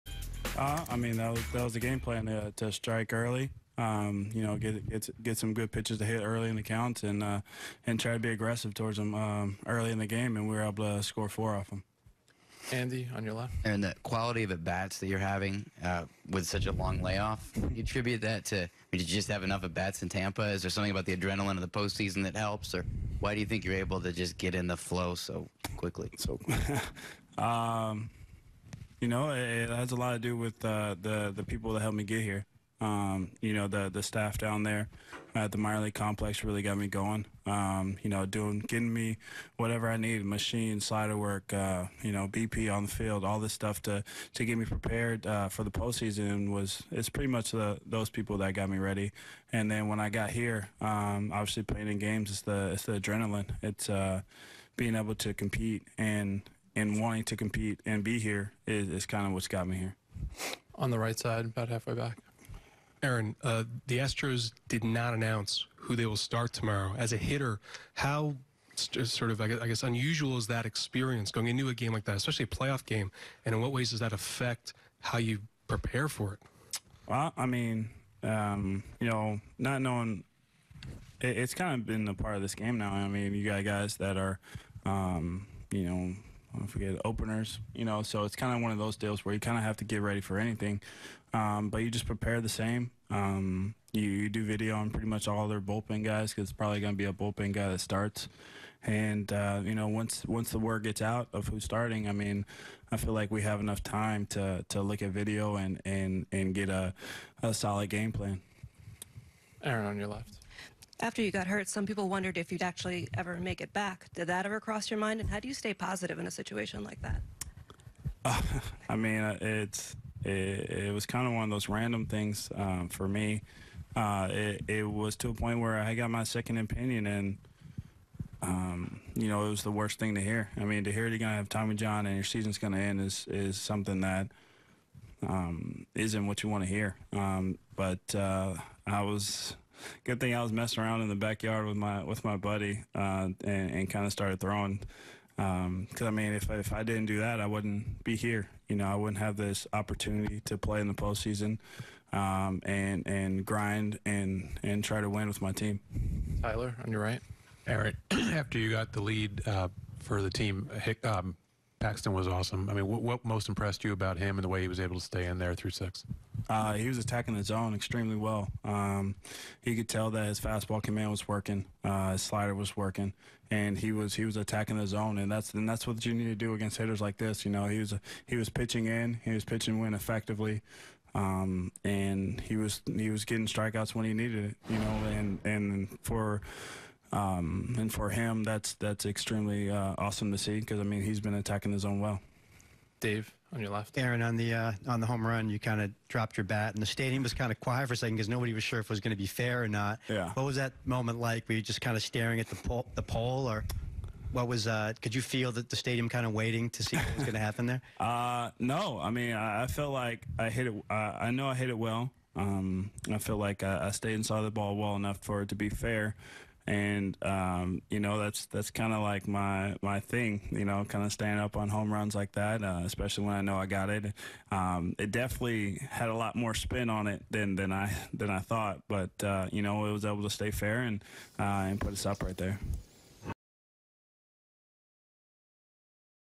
Aaron Hicks on Yankees ALCS Game 5 victory
Aaron Hicks: Good thing I was messing around in the backyard with my buddy ... Otherwise, I wouldn't be here right now.